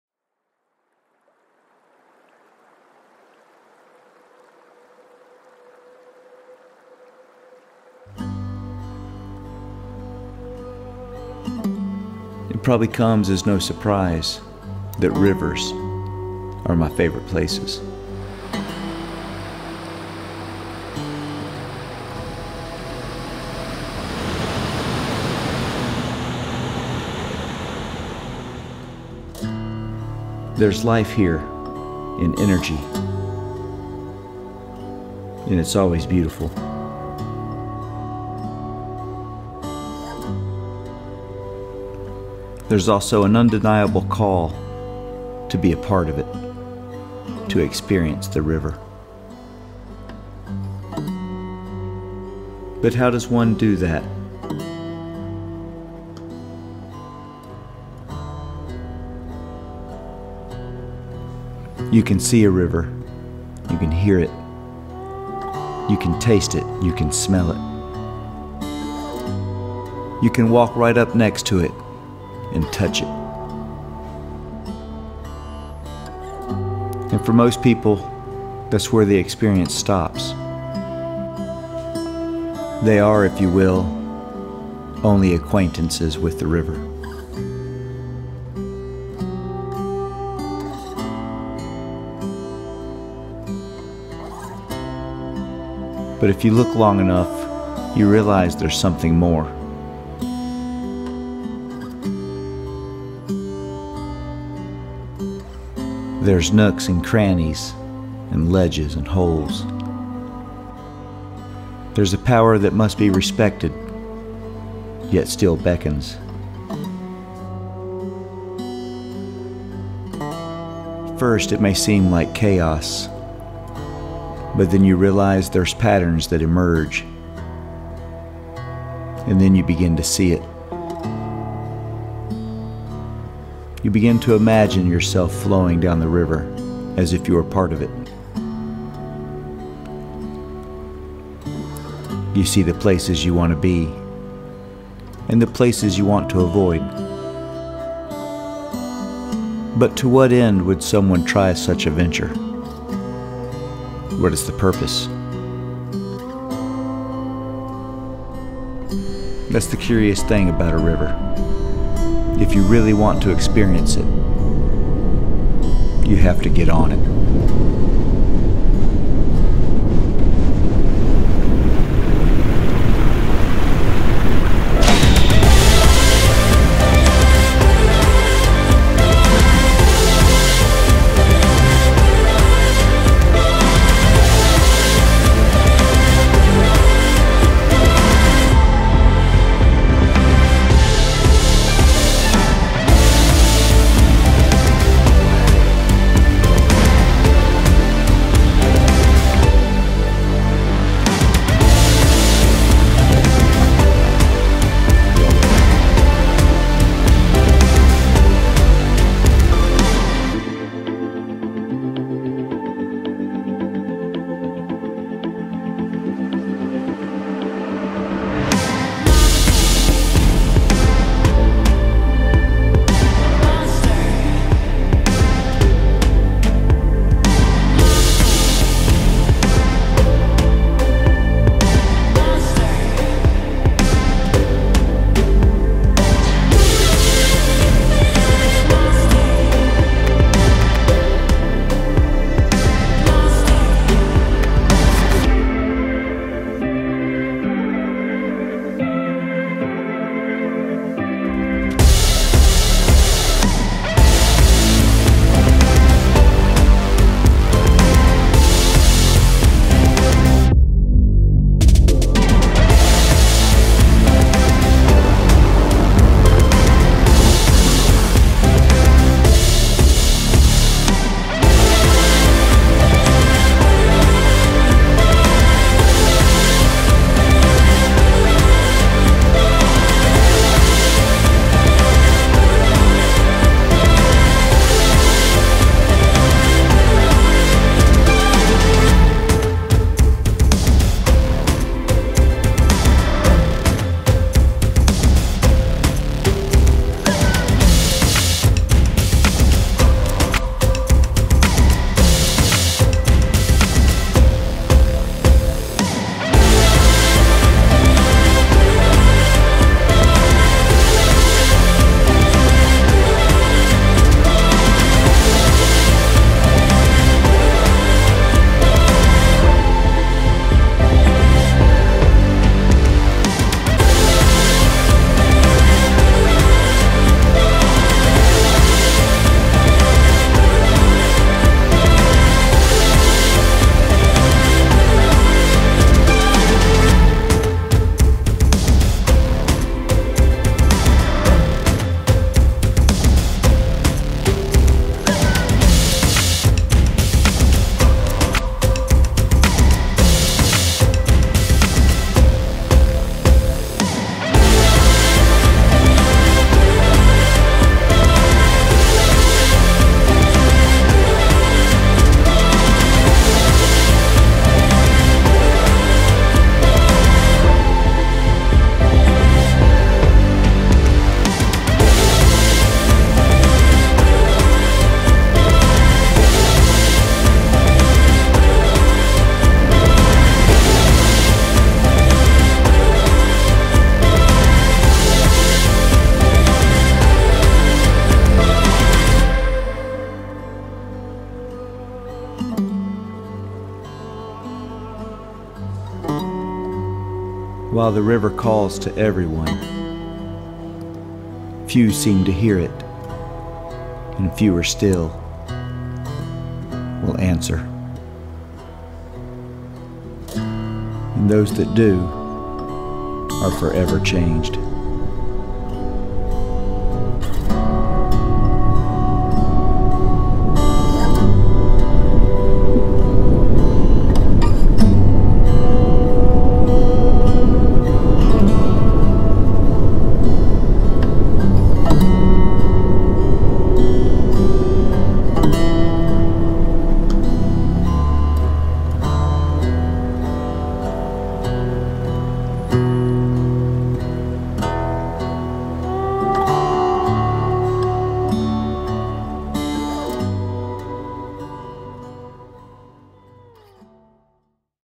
A Day on the River - Tellico Ledges
Come along and experience a day on the river. If you truly want to know a river, you must get on the river.
Had a blast making this one folks!  Other than topic based projects, I usually dont have a predetermined "script" for making a video or how that video turns out.  Rather I just start it and see where it takes me.  This one did not go as I had planned...
Back in early spring, I had another wonderful opportunity to paddle and film with Ken Whiting of Go Paddle and Paddle TV.  If you dont know, he is a class act and true ambassador to the sport of paddling - not just kayaking (of which he is a world champion play boater).  He covers virtually all disciplines of the sport and showcases them all.  On a recent project in Tennessee, he reached out looking for ideas on locations to film.  There were a few obvious choices but none better for what he was looking to do than the Tellico.  He and camera Ninja Mat Willder met P5, Austin Wallis and I for a fabulous day showcasing the Tellico Ledges.  Austin and Mat spent the day following us down the river grabbing all the footage as Ken, P5 and I pretended to be "working" - code for we hucked ourselves off all the ledges and drops that this kayaker's playground has to offer.  Once again, Ken made all of his footage available to me.  As a content creator, this makes me feel like a kid in a candy store!!!   It was sooo easy to make this video!  Enjoy!  Thanks Ken and Matt for such a fun day and including us in your project!